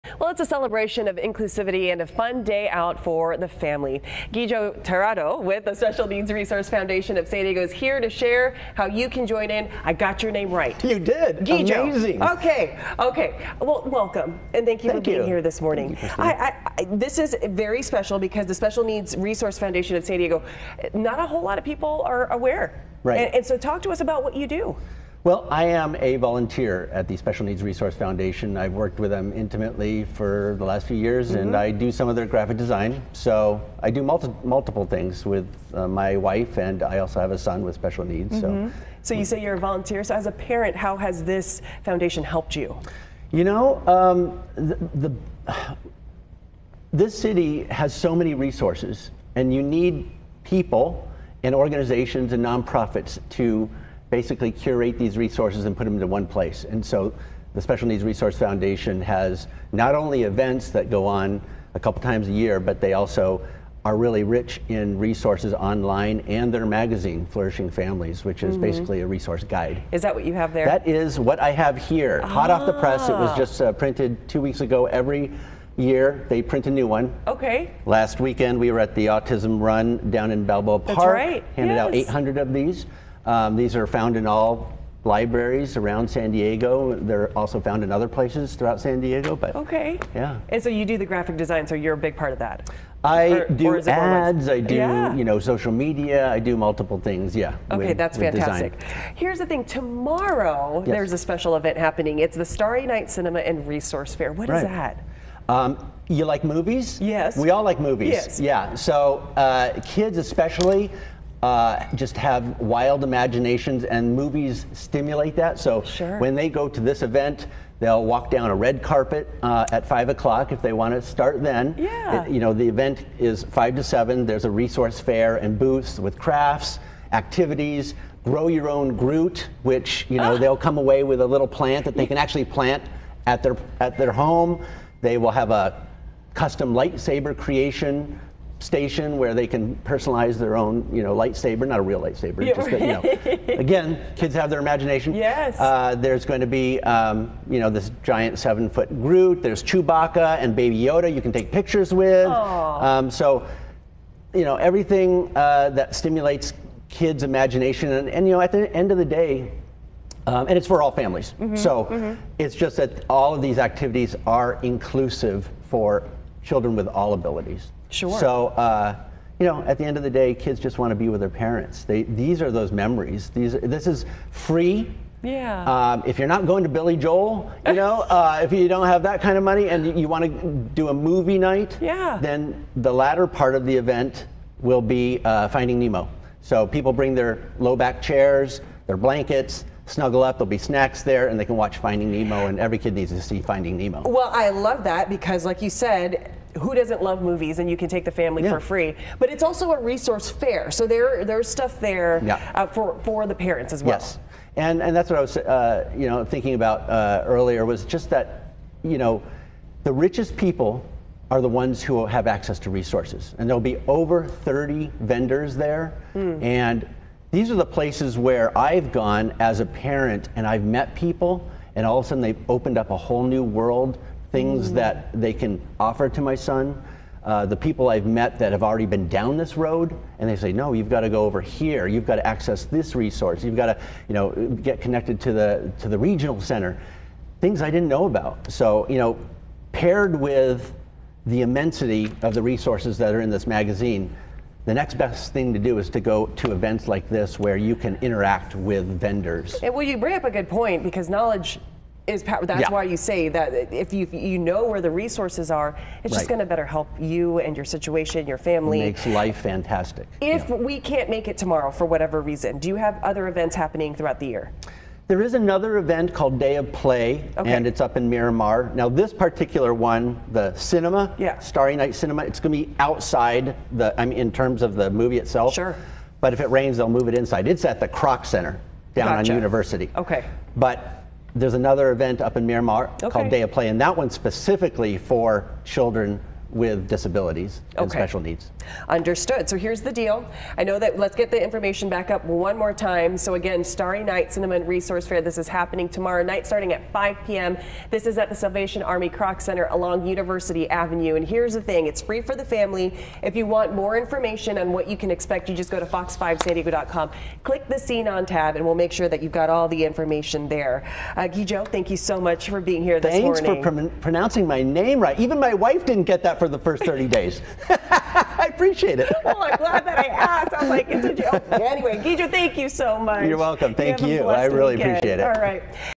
Special Needs Starry Night Cinema & Resource Fair
Gijo Tirado talks about being a parent of a special needs child and the resources in San Diego that can help.